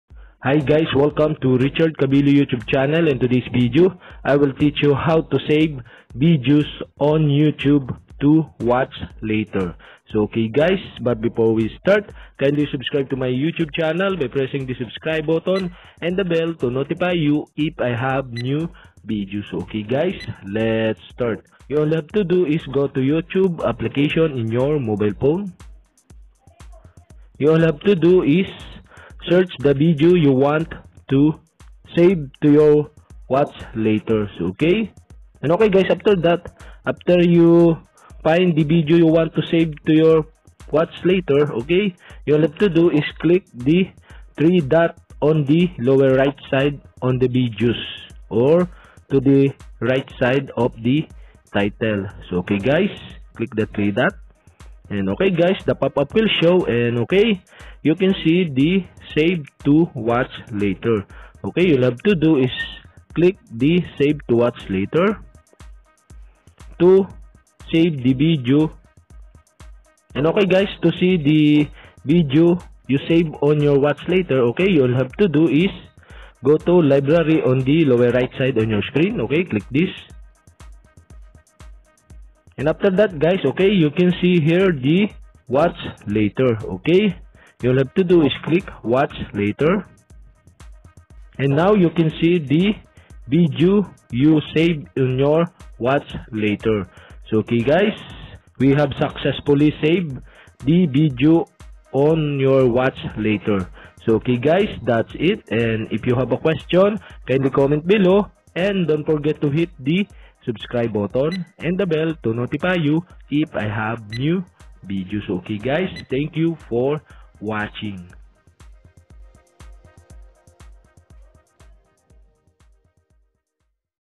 How to Save Videos on YouTube to Watch Later
How to put videos into watch later on YouTube. In this tutorial I will teach you how to save videos on youtube to watch later.

Thanks...